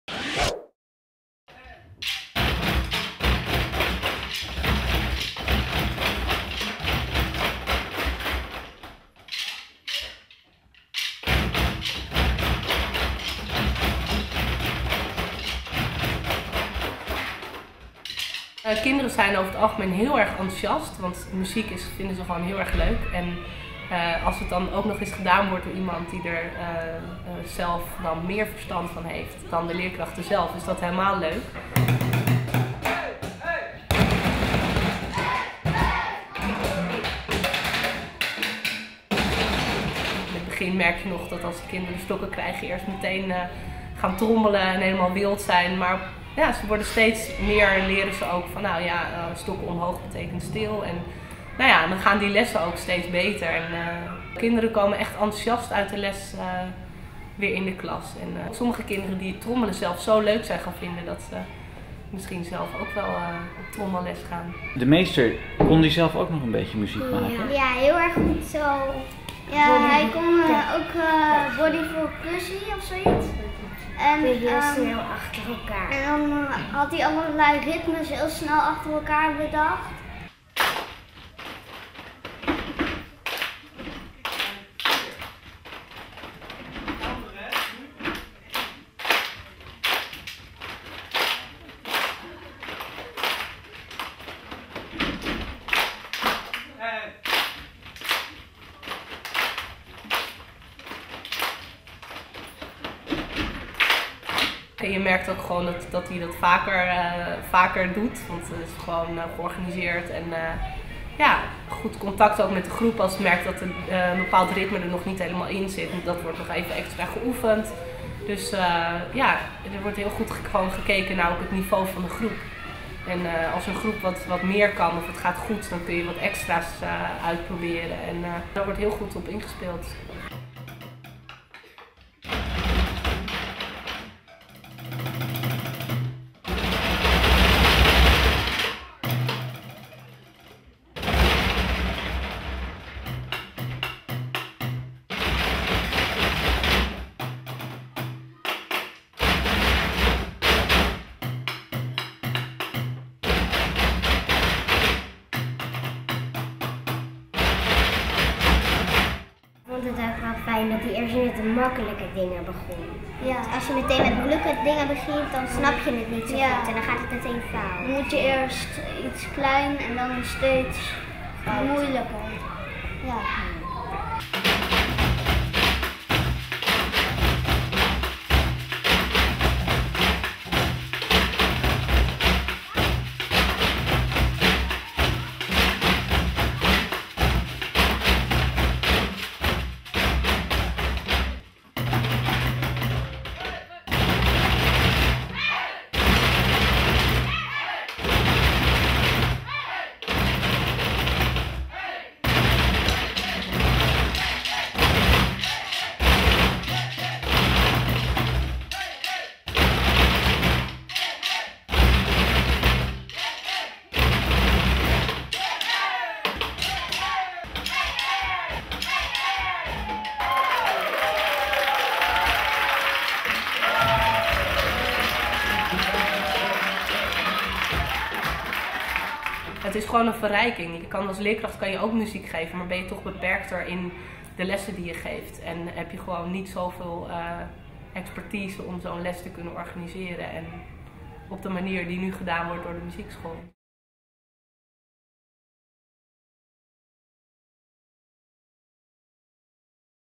MSA trommel op rommel Olaf keus , school Amsterdam
Muziek school Amsterdam , project trommel op Rommel door Olaf Keus  op de 8Hoek in Amsterdam 2009